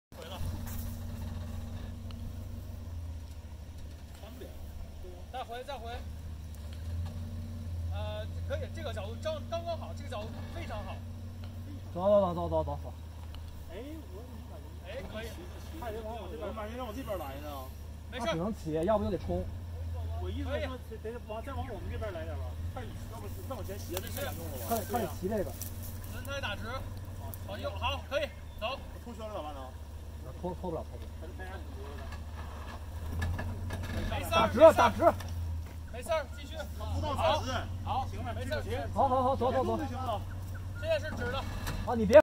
墨尔本Vlog 华人户外运动 越野 —越域4WD— 墨尔本最猛素车帕杰罗 第三段 素车也是很猛的 无兄弟不越野
墨尔本Vlog 华人户外运动 越野 —越域4WD— 墨尔本最猛素车帕杰罗